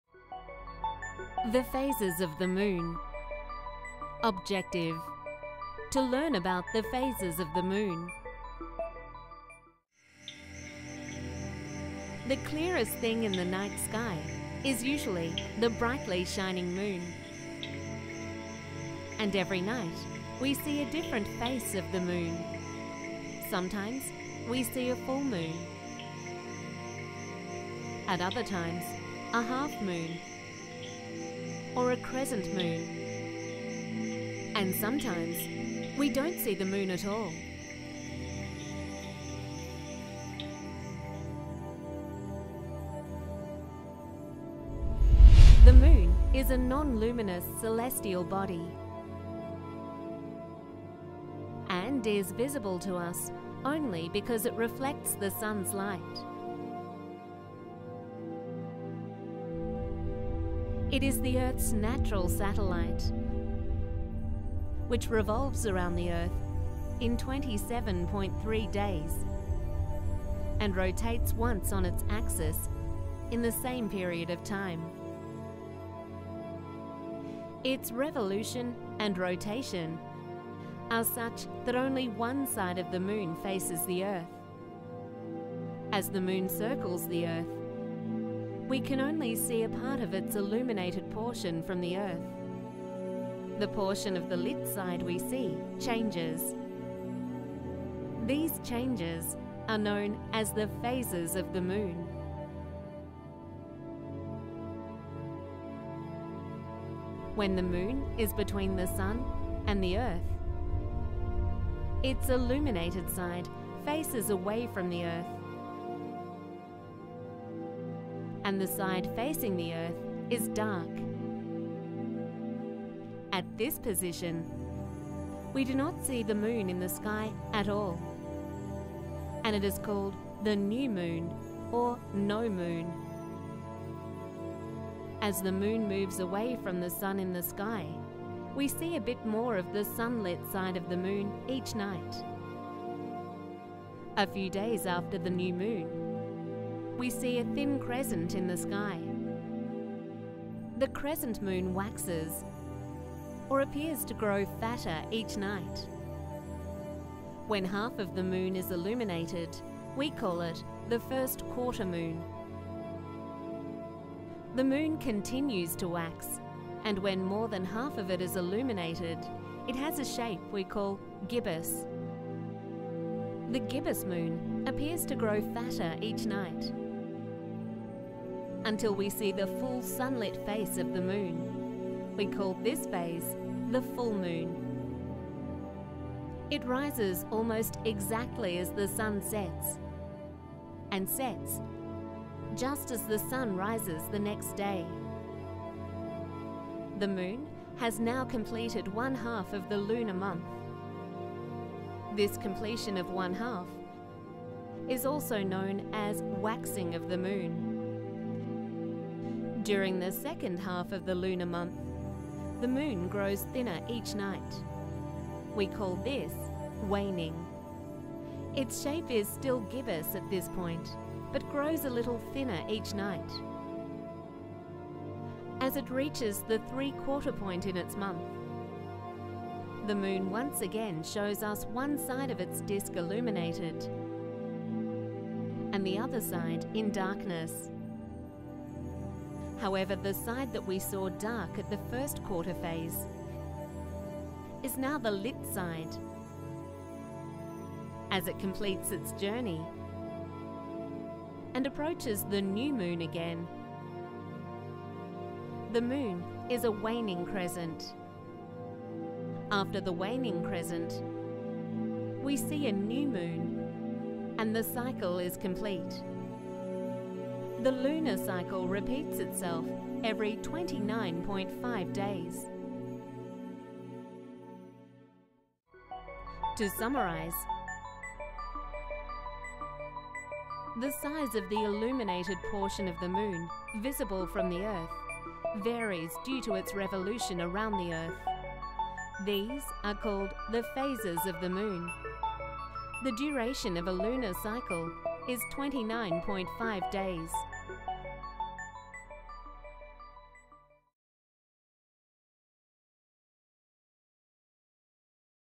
The Phases Of The Moon
Access lesson resources for this video + more elementary solar system videos for free on ClickView ✅

This video explores the appearance, causes and effects of the different phases of the moon and is a perfect introduction for students.